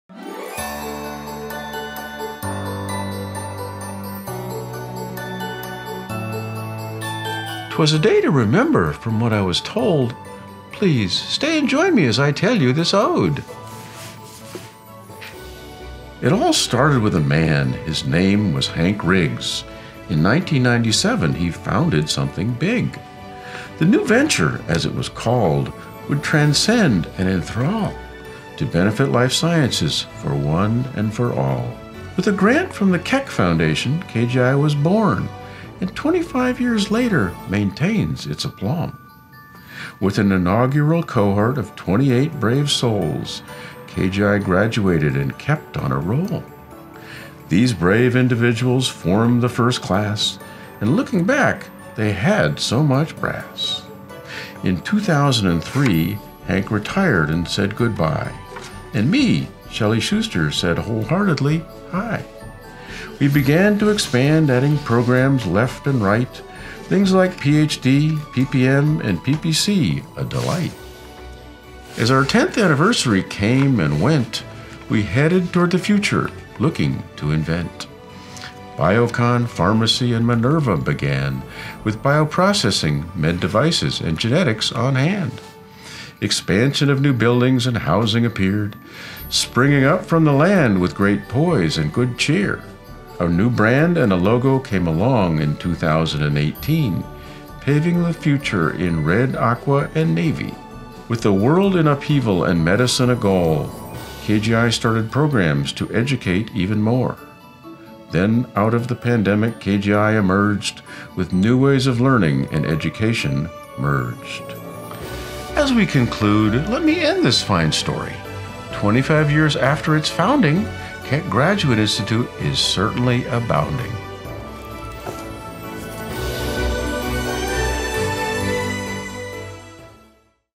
Happy Holidays From KGI 2022
As we cherish the moments of our first 25 years, we want to acknowledge how grateful we are for our faculty, staff, alumni, trustees, corporate partners, preceptors, and friends. Your support is key to the success of KGI. We wish you a festive holiday season and a new year of health and happiness.